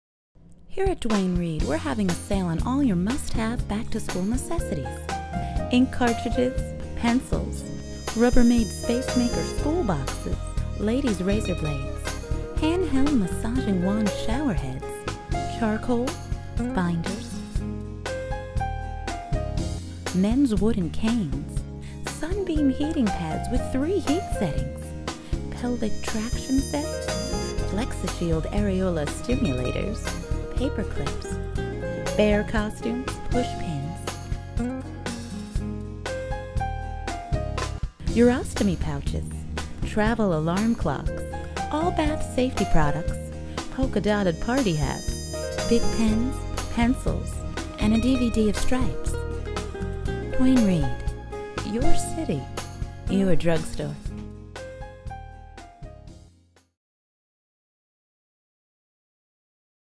Back to School with Duane Reade
Duane Reade has all your back to school deals! ALL OF THEM.
Created for the September 14th Gorilla Gorilla show at the UCB Theatre in NY.

Directed by Eric Drysdale
Written by Jill Morris
Edited by Ramsey Ess
Narration by Jen Bartels
Photos by Ari Scott
Produced by Amey Goerlich